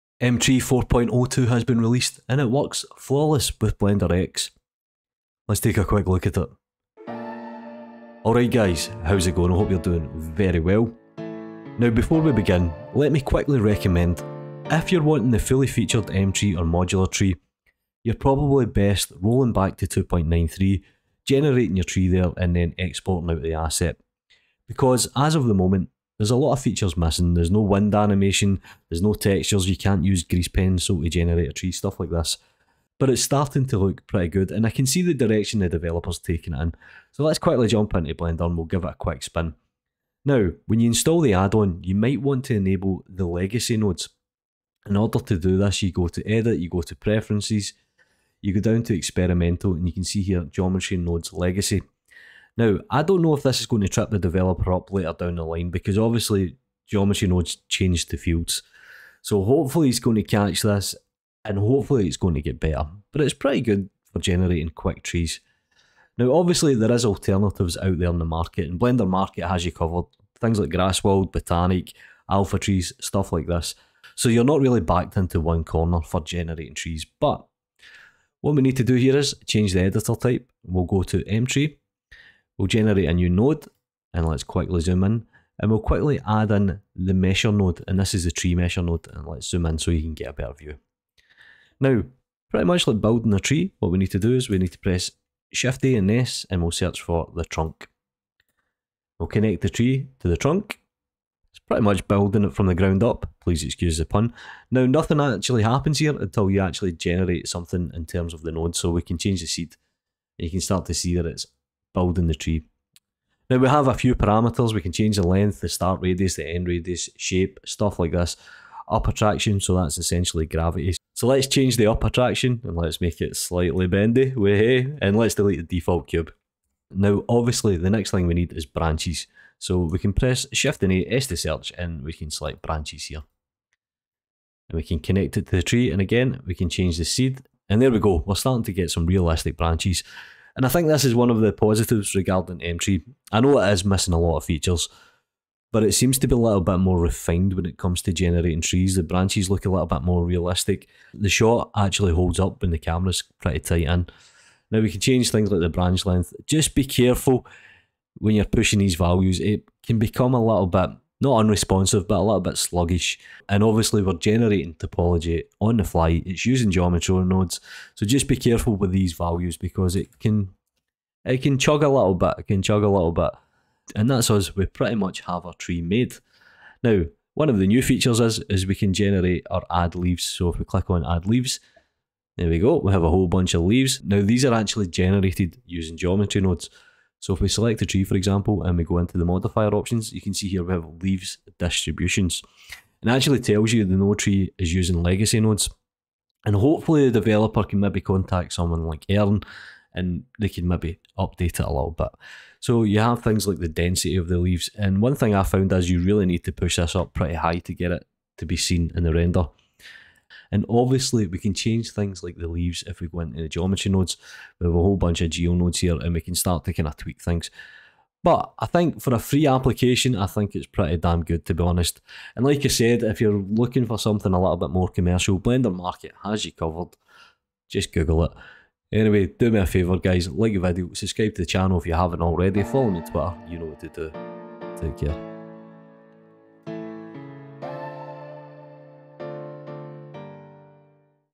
Mtree 4.0.2 for Blender 3.X
Mtree is receiving some love for Blender 3.0 here's a quick look at the development.

blender addon free update mtree free blender plugin generate trees